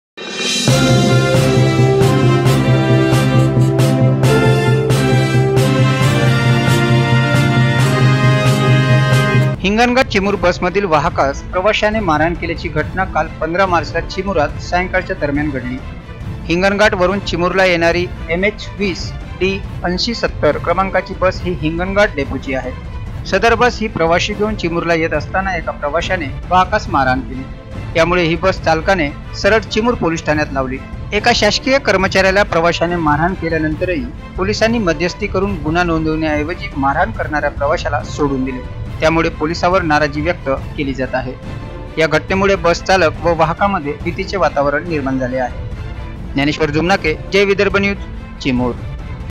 प्रवाशाची बसवाहकास मारहाण
चिमूर येथील घटना: पोलिसांनी मारहाण करणा-या प्रवाशास सोडले